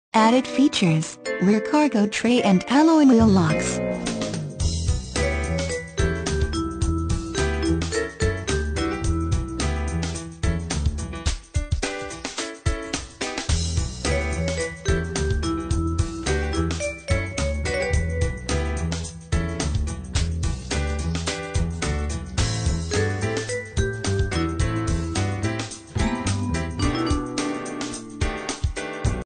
2012 HYUNDAI GENESIS Lindon, UT GE3735
2012 HYUNDAI GENESIS Lindon, UT
Stock# GE3735

425 S. Lindon Park Dr

Added Features: Rear Cargo Tray and Alloy Wheel Locks.